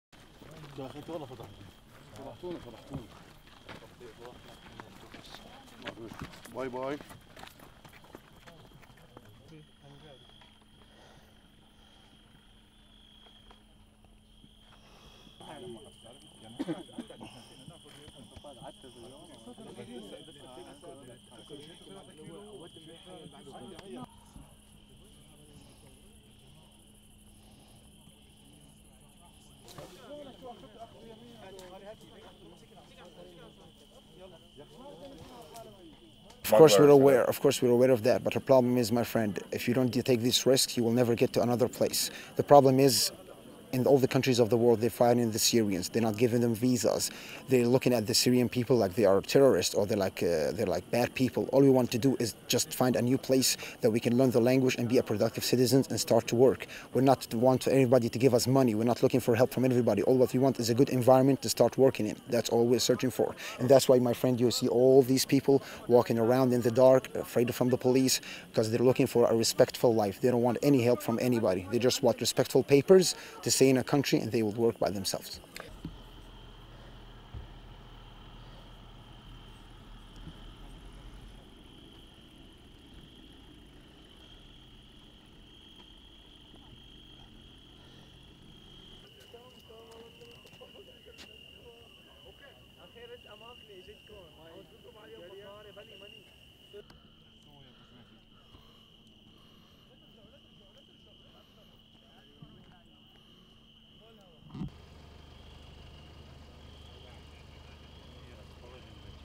Police patrol border area as migrants arrive
(31 Aug 2015) Migrants continued to approach the Hungarian border from Serbia on Monday morning.
The cornfields around the northern Serbian town of Horgos are filled with hundreds of mostly Syrian refugees trying to illegally cross into Hungary, and the European Union.
The Hungarian army has been deployed at a barbed wire fence on the border in attempt to prevent the migrants from crossing.